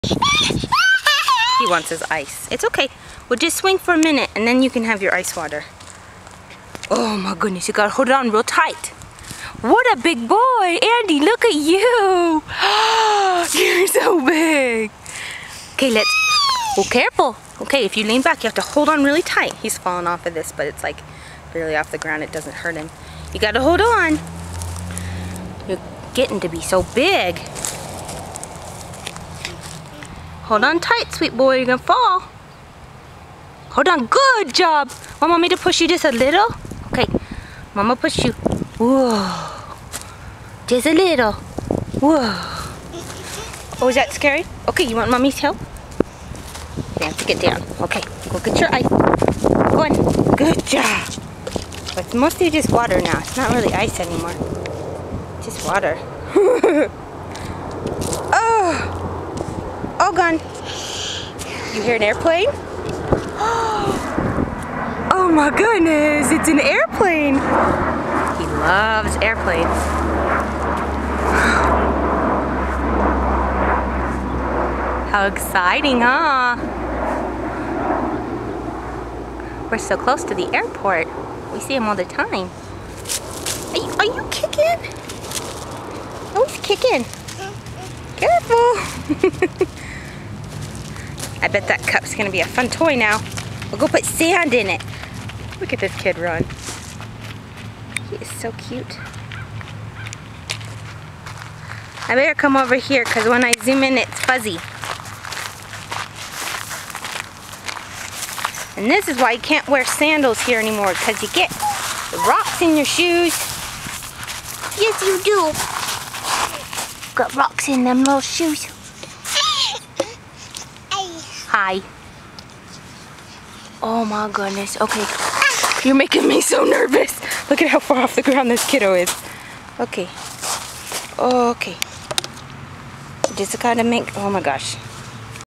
Toddler learns to swing
This is Andy, 1 1/2 years, playing at the park in June 2011. This was the first time he learned to swing "like a big boy" and he was so proud of himself.